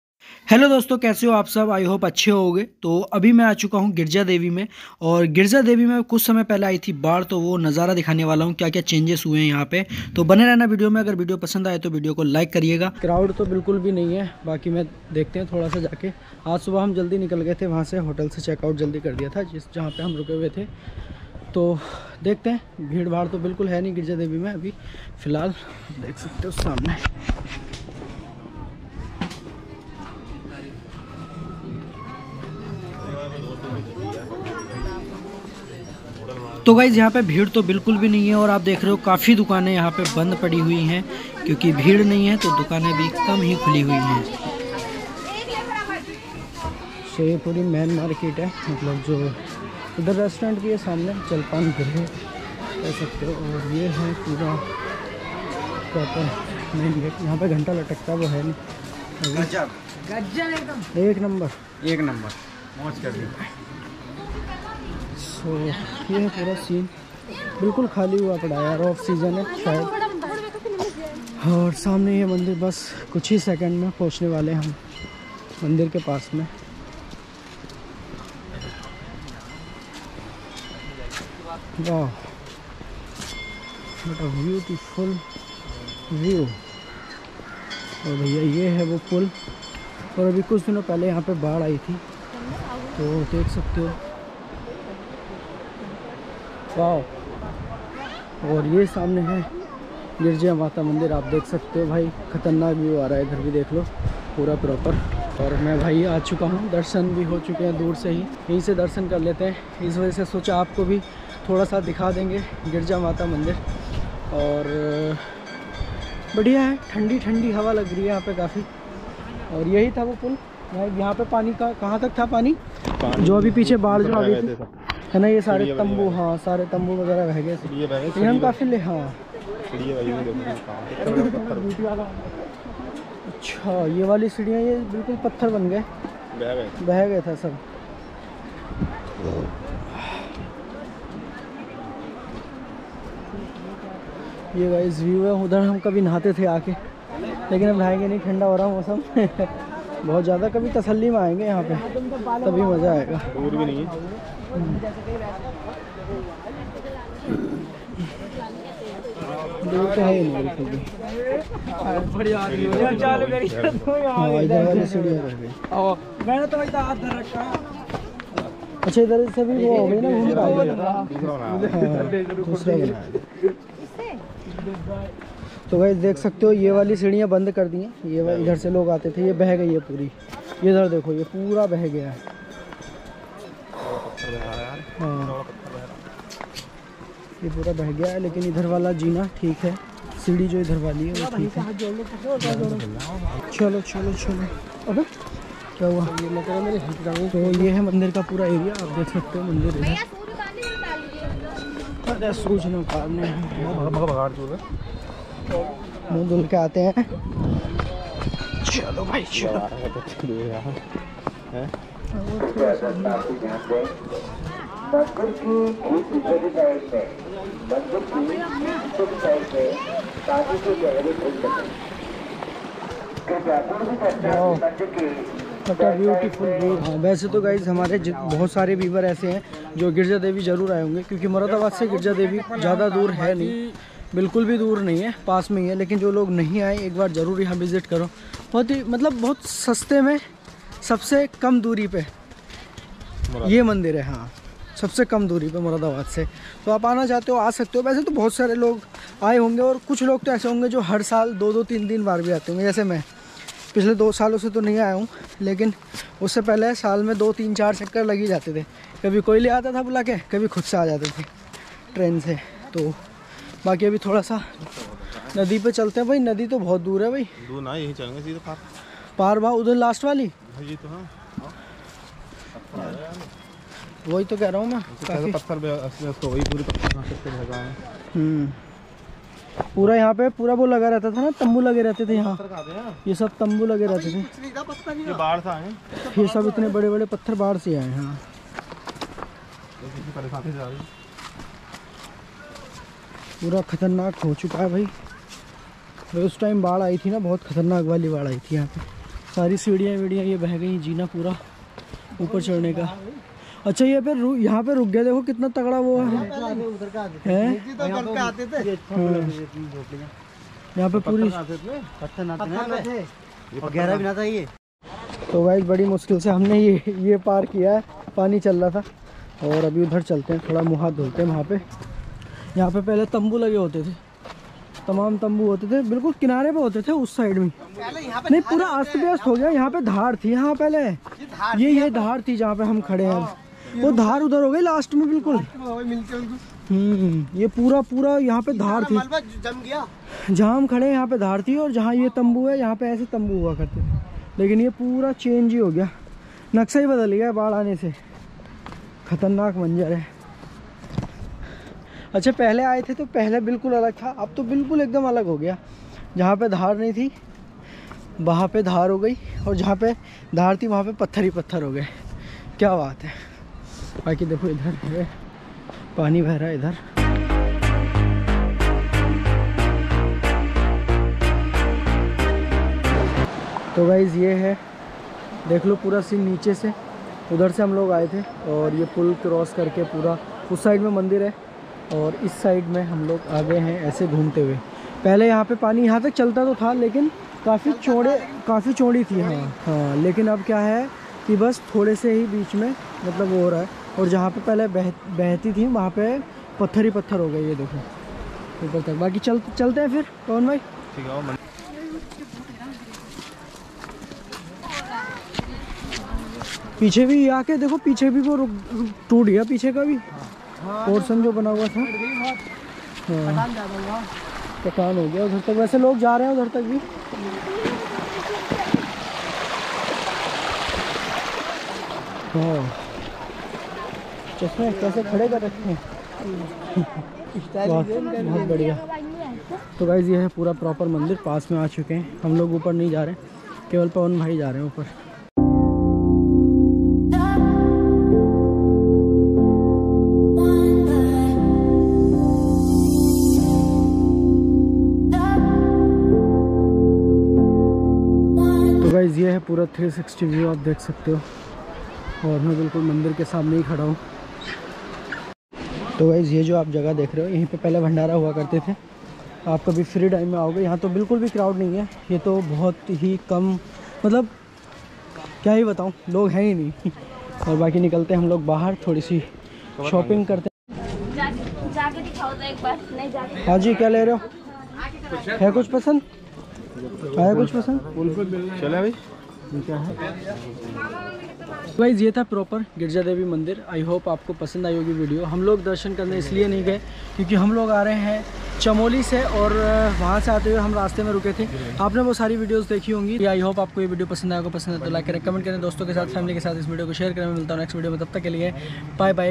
Girija Mata Mandir Vlog 2022 || बाढ़ के बाद का खतरनाक हाल || Ramnagar Uttrakhand 2022
Vlogging Gear
Go pro hero 9
boya mic m1
boya mic mm1
my phone
memory card 64GB
memory card128GB